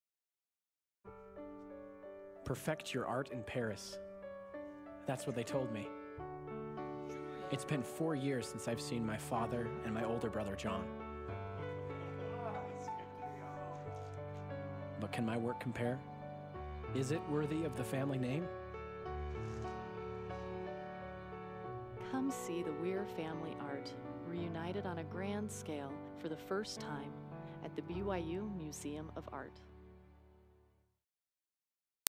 Meet the Weirs
The Weir Family, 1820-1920: Expanding the Traditions of American Art, organized by the Brigham Young University Museum of Art, is the first major exhibition to collectively examine paintings by Robert Walter Weir, John Ferguson Weir, and Julian Alden Weir. It showcases important pieces from museum and private collections across the country, many of which have never been seen together. The exhibition will explore how the transatlantic encounters of the Weir family of artists helped to shape American art for nearly a century.